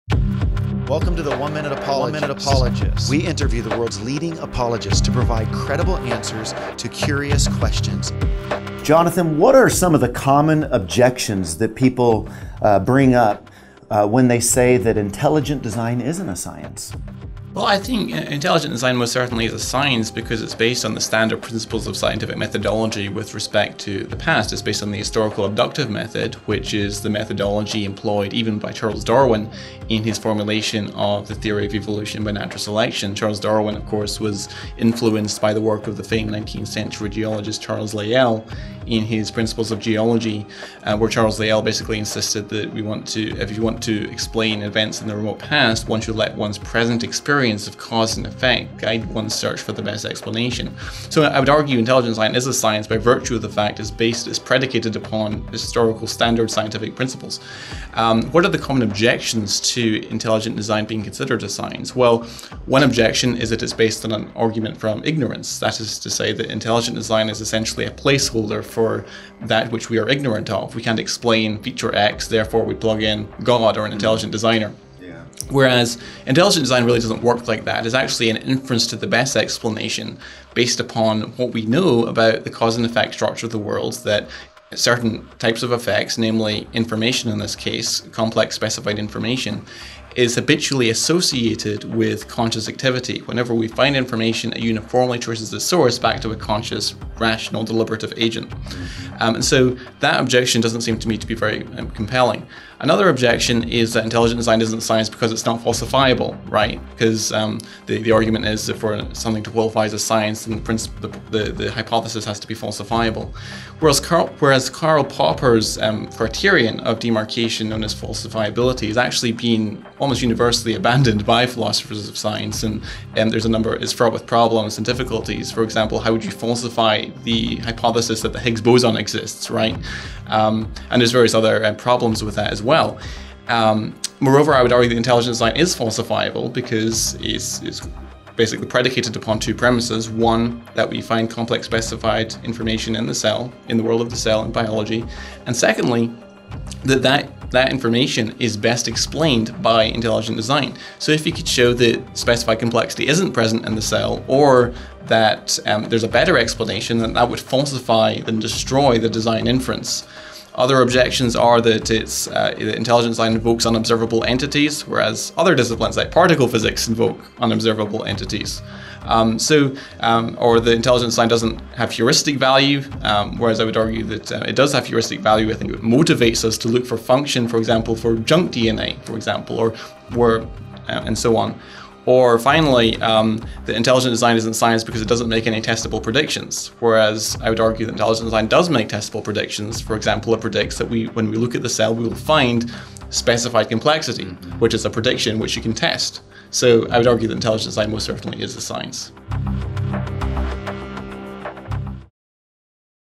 794. Is Intelligent Design A Science?
Jonathan McLatchie responds to those who claim that intelligent design is not a science.